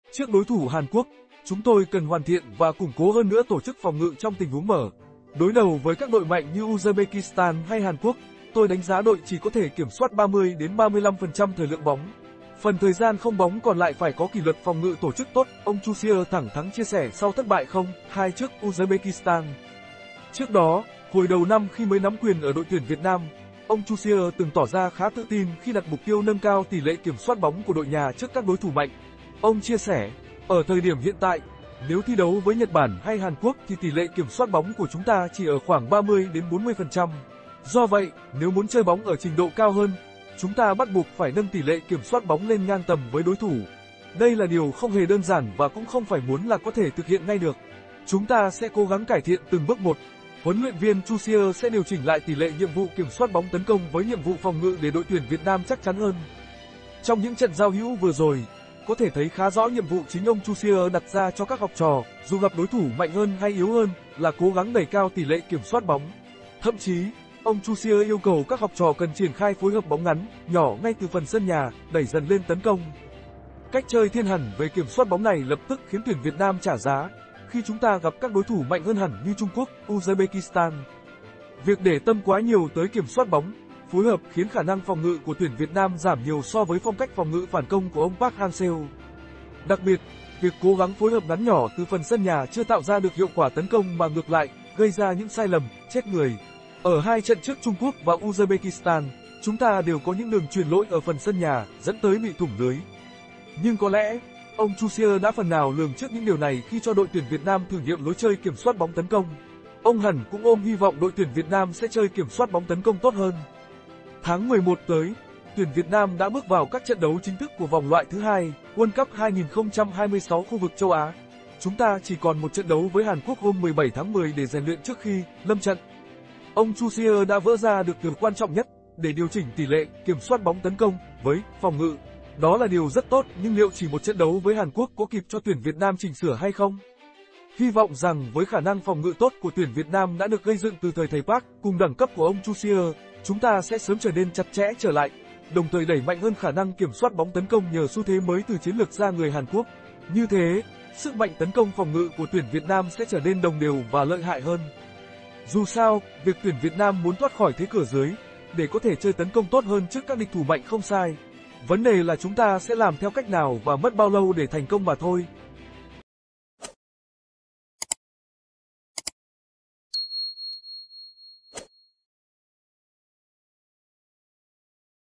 VIỆT NAM CÓ KỊP 'LỘT XÁC' TRƯỚC HÀN QUỐC KHI ĐÂY LÀ TRẬN GIAO HỮU CUỐI CỦA HLV TROUSSIER TRƯỚC VÒNG
"Trước đối thủ Hàn Quốc, chúng tôi cần hoàn thiện và củng cố hơn nữa tổ chức phòng ngự trong tình huống mở. Đối đầu với các đội mạnh như Uzbekistan hay Hàn Quốc, tôi đánh giá đội chỉ có thể kiểm soát 30-35% thời lượng bóng. Phần thời gian không bóng còn lại phải có kỷ luật phòng ngự tổ chức tốt"  – ông Troussier thẳng thắng chia sẻ sau thất bại 0-2 trước Uzbekistan.